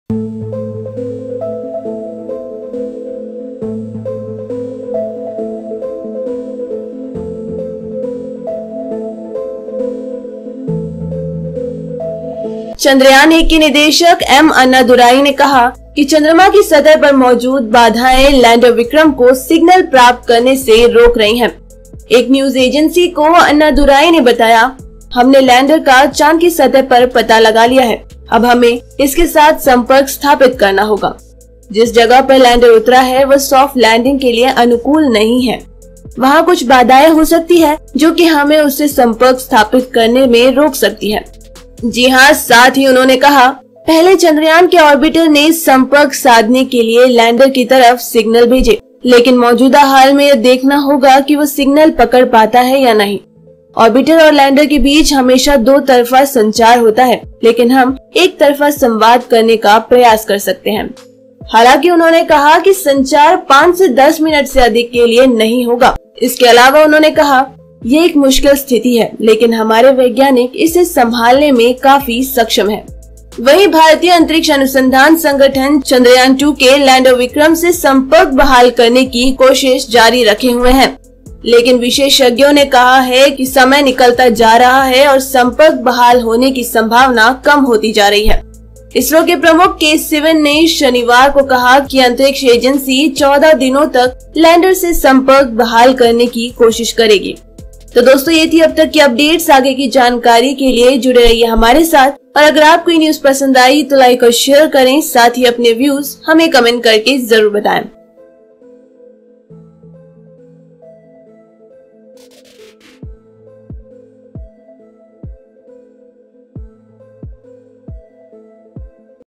चंद्रयान -1 के निदेशक एम. अन्नादुराई ने बताया लैंडर विक्रम से क्यों नहीं हो रहा सम्पर्क?
Why ISRO Lost Contact With Moon Lander - Chandrayaan 1 Director's Theory | ISRO chairman K Sivan said that the space agency has found the location of the Vikram Lander through a thermal image clicked by the Chandrayaan 2 Orbiter, which is already in the intended orbit around the Moon.

EDITOR ►